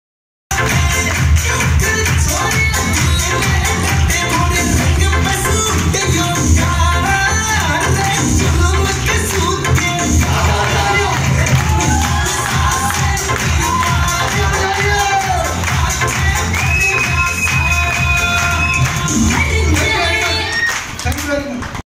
Safe shop celebrating | Vijay Utsav
I attended this meeting on 4th April 2021 in jhansi.